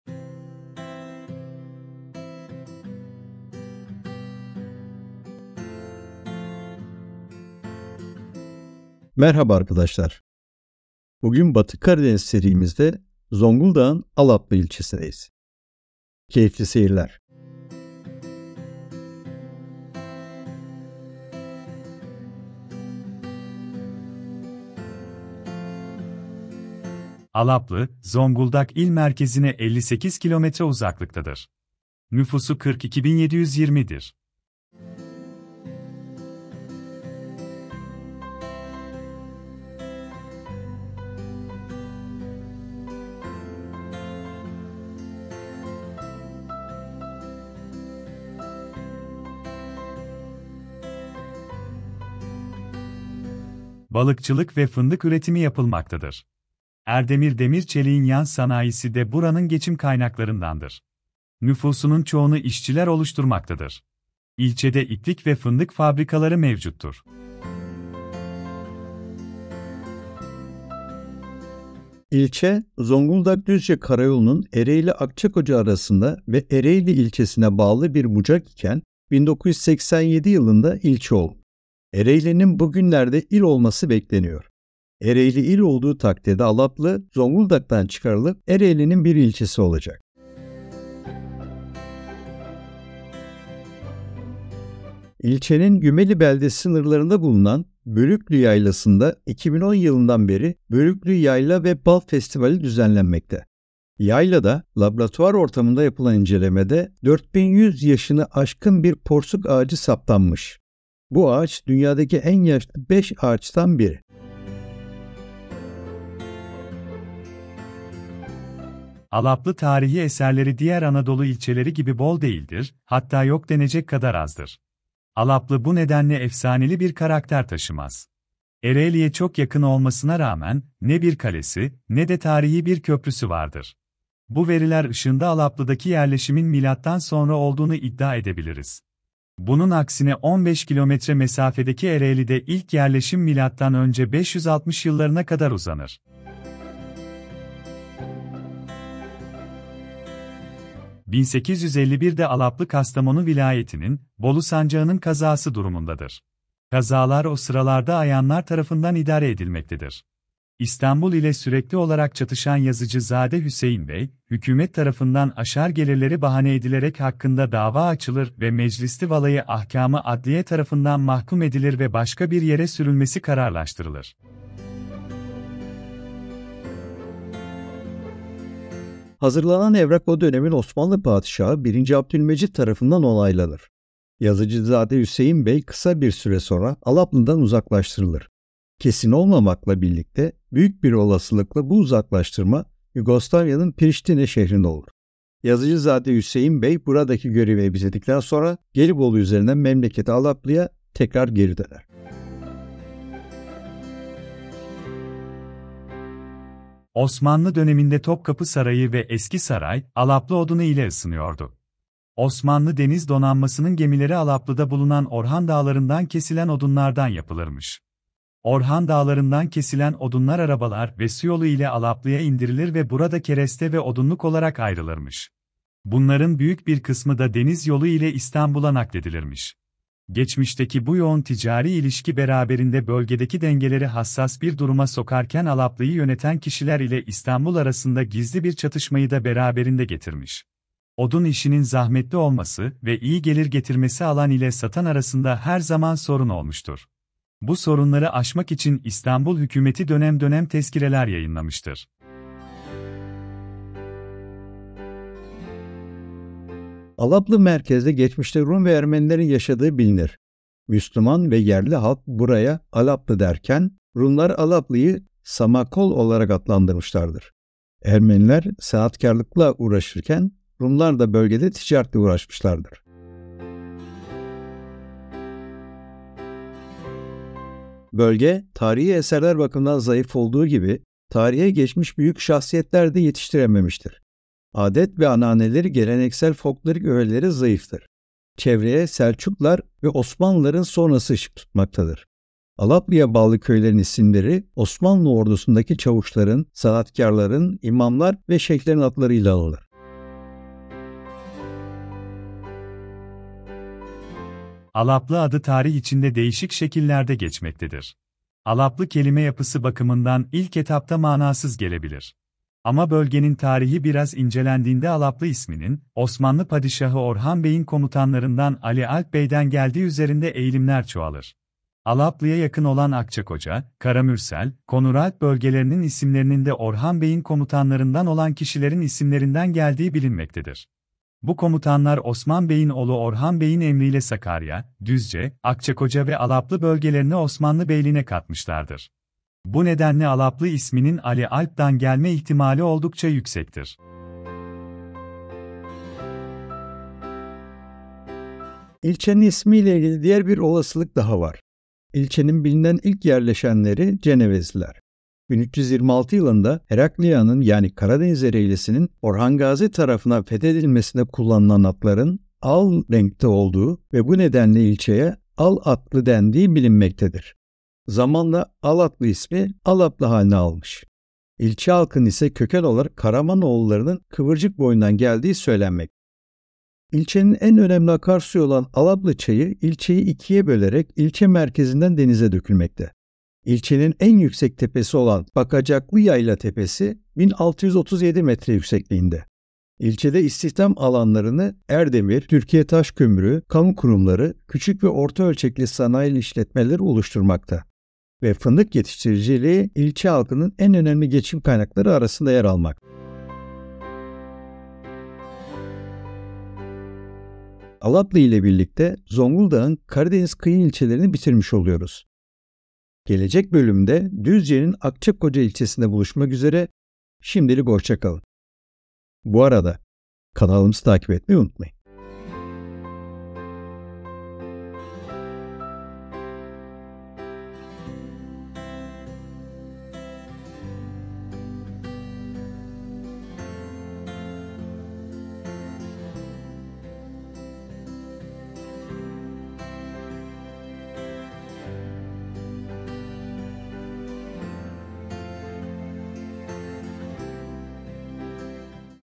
ALAPLI Zonguldak - Batı Karadeniz Kıyı İl ve İlçe Merkezleri 14
Balıkçılık ve fındık üretimi yapılmaktadır. Erdemir Demir Çelik’in yan sanayisi de buranın geçim kaynaklarındandır. Nüfusunun çoğunu işçiler oluşturmaktadır. İlçede iplik ve fındık fabrikaları mevcuttur.
İlçe Zonguldak-Düzce karayolunun Ereğli-Akçakoca arasında ve Ereğli İlçesine bağlı bir bucak iken 1987 yılında ilçe olmuştur. Ereğli’nin bugünlerde İl olması bekleniyor. Ereğli il olduğu takdirde Alaplı Zonguldak’tan çıkarılıp Ereğli’nin bir ilçesi olacaktır.
İlçe sınırlarında bulunan Gümeli Beldesi sınırlarında bulunan Bölüklü Yaylası'nda 2010 yılından beri Bölüklü Yayla ve Bal Festivali düzenlenmektedir. Yaylada laboratuvar ortamında yapılan incelenmede saptanan 4.100 yaşını aşkın bir porsuk ağacı bulunmaktadır. Bu ağaç dünyadaki en yaşlı 5 ağacından biridir.
1851`de Alaplı Kastamonu Vilayetinin, Bolu Sancağının Kazası durumundadır. Kazalar o sıralarda ayanlar tarafından idare edilmektedir. İstanbul ile sürekli olarak çatışan Yazıcı Zade Hüseyin Bey, hükümet tarafından aşar gelirleri bahane edilerek hakkında dava açılır ve Meclis-i Vala-yı Ahkam-ı Adliye tarafından mahkum edilir ve başka bir yere sürülmesi kararlaştırılır.
Hazırlanan evrak o dönemin Osmanlı Padişahı 1. Abdülmecid tarafından onaylanır. Yazıcı Zade Hüseyin Bey kısa bir süre sonra Alalplı' dan uzaklaştırılır. Kesin olmamakla birlikte büyük bir olasılıkla bu uzaklaştırma Yugoslavya`nın Priştine şehrine olur. Yazıcı Zade Hüseyin Bey buradaki görevini bitirdikten sonra Gelibolu üzerinden memleketi Alaplı’ya tekrar geri döner.
Osmanlı döneminde Topkapı Sarayı ve Eski Saray, Alaplı odunu ile ısınıyordu. Osmanlı Deniz Donanması'nın gemileri Alaplı' da bulunan Orhan Dağları ve bu sıradağa bağlı dağlardan kesilen odunlardan yapılmıştır. Orhan Dağları ve bu sıradağa bağlı dağlardan kesilen odunlar, arabalar ve suyolu ile Alaplı’ya indirilir ve burada, kereste ve odunluk olarak ayrılırmış. Bunların büyük bir kısmı da deniz yolu ile Istanbul'a nakledilirmiş.
 Geçmişteki bu yoğun ticari ilişkisi beraberinde bölgedeki dengeleri hassas bir duruma sokarken, Alaplı’yı yöneten kişiler ile İstanbul arasında gizli bir çatışmayı da beraberinde getirmiş. Odun işinin zahmetli olması ve iyi gelir getirmesi alan ile satan arasında her zaman sorun olmuştur. Bu sorunları aşmak için İstanbul Hükümeti dönem dönem tezkireler yayınlamıştır.
Alaplı merkezde geçmişte Rum ve Ermenilerin yaşadığı bilinir. Müslüman ve yerli halk buraya Alaplı derken, Rumlar Alaplı'yı Samakol olarak adlandırmışlardır. Ermeniler sanatkârlıkla uğraşırken, Rumlar'da bölgede ticaretle uğraşmışlardır.
Bölge tarihi eserler bakımından zayıf olduğu gibi, tarihe geçmiş büyük şahsiyetler de yetiştirememiştir. Adet ve ananeleri geleneksel, folklorik ögeleri zayıftır. Çevreye Selçuklular ve Osmanlıların sonrası ışık tutmaktadır. Alaplı'ya bağlı köylerin isimleri, Osmanlı Ordusu'nda ki çavuşların, sanatkârların, imamlar ve şeyhlerin adlarıyla anılır.
Alaplı adı tarih içinde değişik şekillerde geçmektedir. Alaplı kelime yapısı bakımından ilk etapta manasız gelebilir. Ama bölgenin tarihi biraz incelendiğinde Alaplı isminin, Osmanlı Padişahı Orhan Bey'in komutanlarından Ali Alp Bey’den geldiği üzerinde eğilimler çoğalır. Alaplı'ya yakın olan Akçakoca, Karamürsel, Konuralp bölgelerinin ismlerinin de Orhan Beyin komutanlarından olan kişilerin isimlerinden geldiği bilinmektedir. Bu komutanlar Osman Beyin oğlu Orhan Bey'in emriyle Sakarya, Düzce, Akçakoca ve Alaplı bölgelerini Osmanlı beyliğine katmışlardır. Bu nedenle Alaplı isminin Ali Alp dan gelme ihtimali oldukça yüksektir.
İlçenin ismi ile ilgili diğer bir olasılık daha var. İlçenin bilinen ilk yerleşenleri Cenevizlilerdir. 1326 yılında Hereklea’nın Orhan Gazi tarafından fethedilmesinde kullanılan atların “Al” renkte olduğu ve bu nedenle İlçeye Alatlı denildiği bilinmektedir. Zamanla Alatlı ismi Alaplı halini almıştır. İlçe halkının ise köken olarak Karamanoğullarının kıvırcık boyundan geldiği söylenmektedir.
Osmanlı topraklarına katılan Alaplı bölgesine yerleştirilen Türk boylarının kadınları, kırsal alanda çiftçilik yapmışlar, yetiştirdikleri meyve ve sebzeleri Alaplı'nın içinde oturan Ermeni ve Rum'lara satmışlardır. Mallarını satmak için merkeze inen Türk boyları kadınlarını çarşıya sokmamışlar, 'kadınlar pazarı' diye bölünmüş ve yalnız Rum ve Ermeni kadınlarının alışveriş yaptığı yere bırakmışlardır. Bu gelenek günümüzde hala sürmektedir. Hafta içerisinde çarşamba günleri bostanında yetiştirdiği meyve sebzesini köylü kadınları eski pazar yerinde bulunan alanda satmaktadır.
İlçenin en önemli akarsuyu olan Alaplı Çayı İlçeyi ikiye bölerek ilçe merkezinden denize dökülmektedir. İlçenin en yüksek tepesi olan Bakacaklıyayla Tepesi 1637 m yüksekliktedir. İlçede İstihdam alanlarını ERDEMİR, TTK, kamu kurumları, küçük ve orta ölçekli sanayi işletmeleri oluşturmaktadır ve fındık yetiştiriciliği İlçe halkının en önemli geçim kaynakları arasındadır.